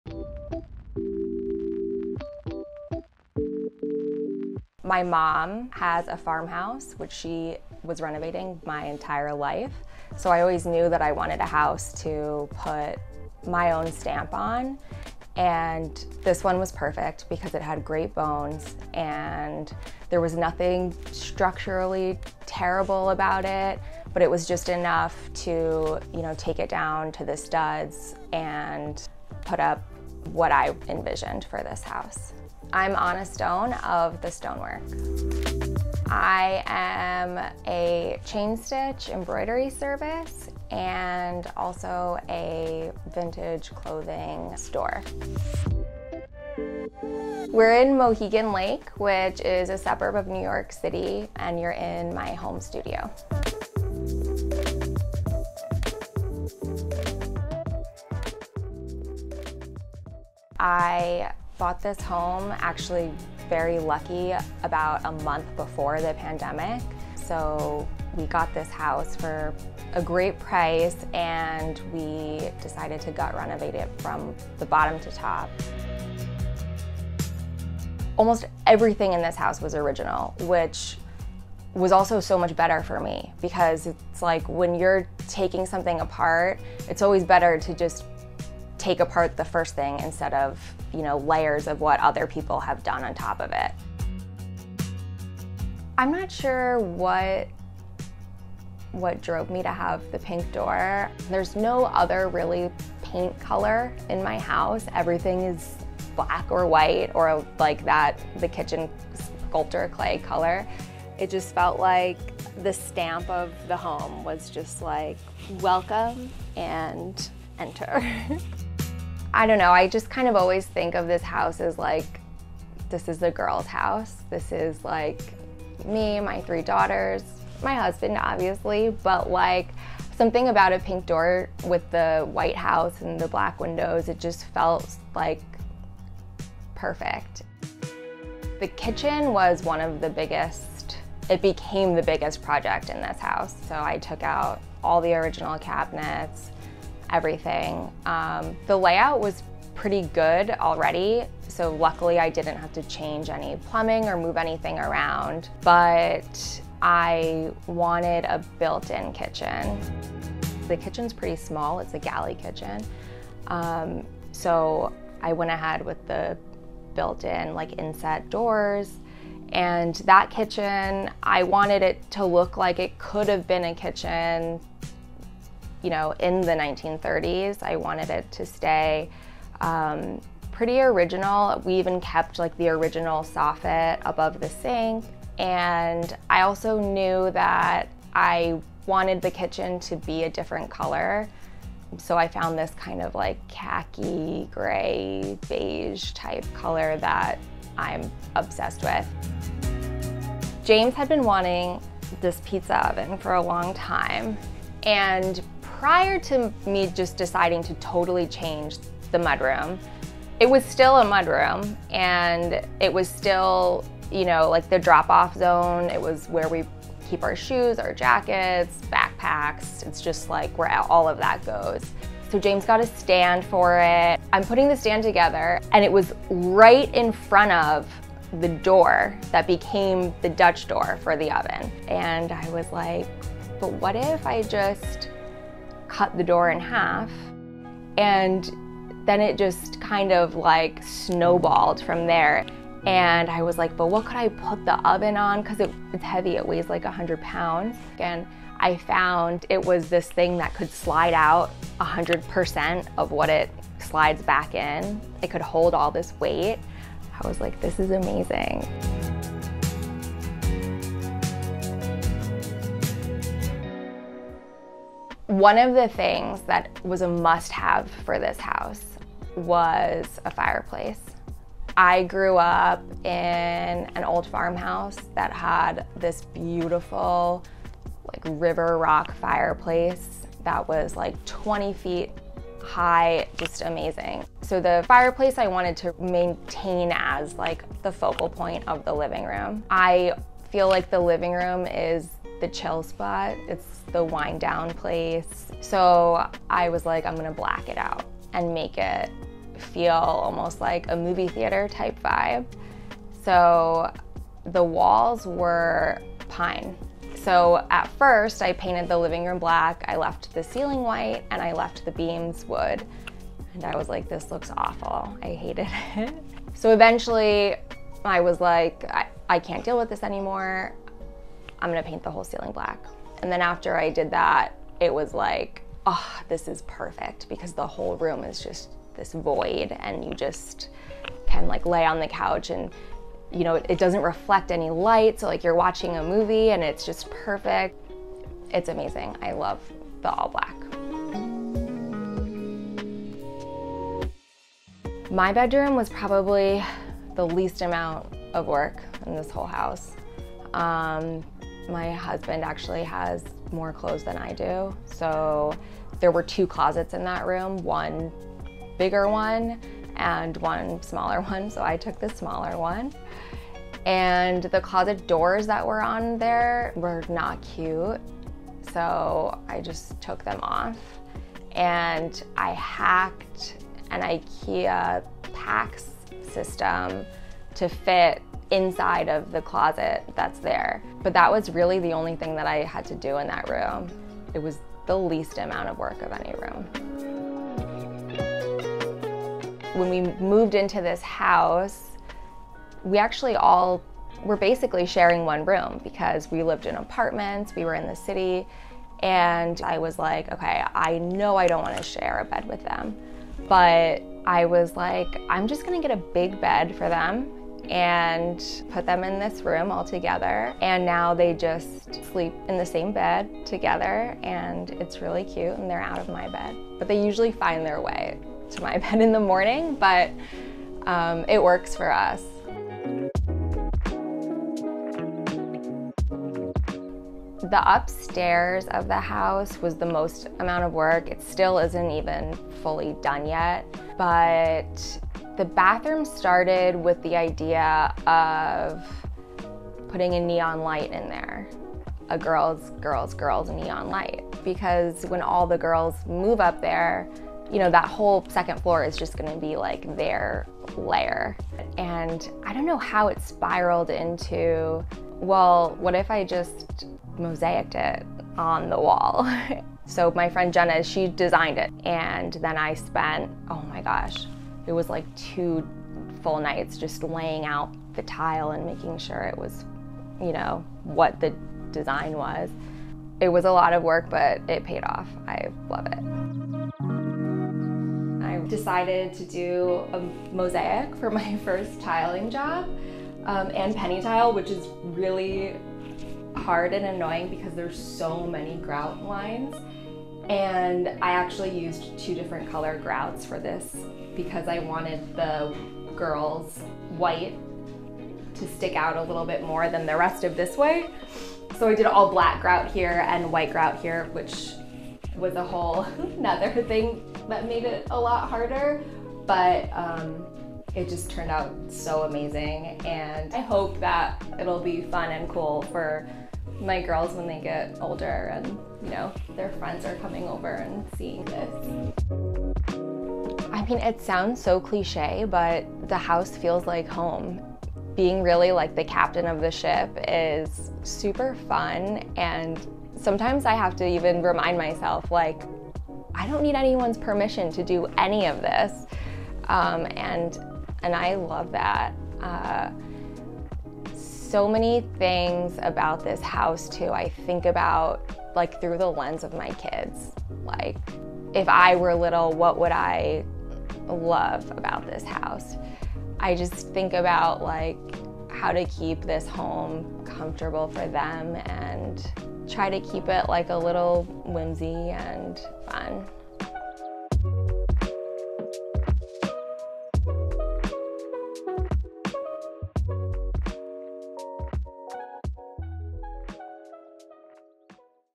Vintage Americana Home Tour of Chain Stitch Embroidery Artist | CREATIVES AT HOME
This week @designerhometours we are in the neighboring NYC lakefront suburb of Mohegan Lake touring the 1933 fully DIY gut-renovated family cape home of celebrity chain stitch embroidery artist Ana Stone of the Stonework.  Ana and her husband James purchased the home at the height of the pandemic, and quickly performed an impressive kid-friendly full DIY gut renovation.  Ana is a master craftsman chain stitch embroiderer, part-owner of an upstate New York vintage clothing store, and experienced antique and vintage collector.  Many of the furnishings in Ana’s home are carefully curated through a combination of Facebook Marketplace and Craigslist finds antique collectibles, and impressively reimagined pieces that she has repaired and repurposed throughout the space.  The home has a casually cool, updated take on classic vintage americana design aesthetic that is both unique and unfussy.  Notable features of the house include a mudroom renovation turned extension of the kitchen that include a commercial Gozney pizza oven, a converted garage turned home gym, full DIY kitchen renovation, an all black family room with a statement fireplace, IKEA DIY closet hack builds, and an intricate mosaic tiled kids bathroom project.   Check out the links below to shop key pieces from this fun, and whimsical family home.

Chapters:
00:00 - About the Creative
01:01 - About the Project
01:36 - Entryway
02:18 - Kitchen
03:21 - Mudroom
04:25 - Living Room
05:44 - Master Bedroom
06:29 - Kids Bedroom
07:07 - Kids Bathroom
07:56 - How To Mosaic Tile a Kids Bathroom
08:43 - Closing